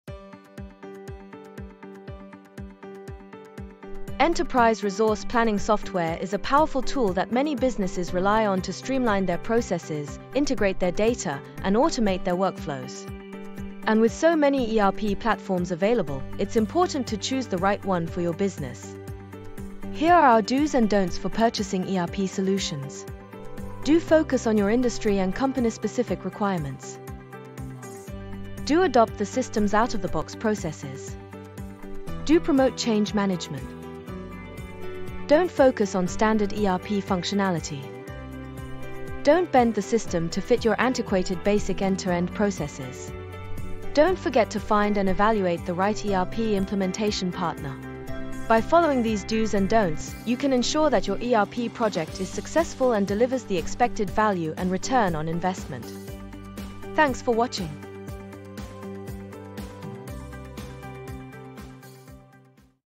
The DOs and DON'Ts of buying ERP software
We know that purchasing new solutions is not a simple decision - that's why this handy video explores the DOs and DON'Ts of buying ERP software, based on our experience and expertise.